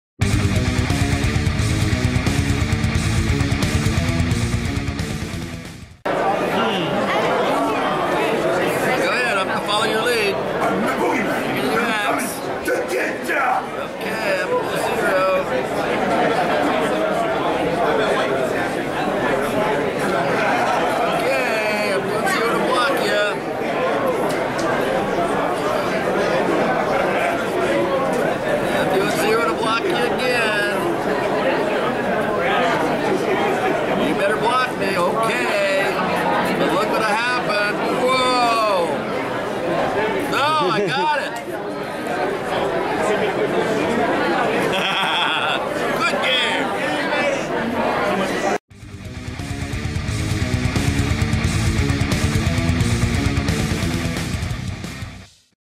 Butch Patrick (Eddie Munster) vs Brad in GAMES WITH FAME!!
Brad battles it out in a game of Tic Tac Toe with Eddie Munster himself!!!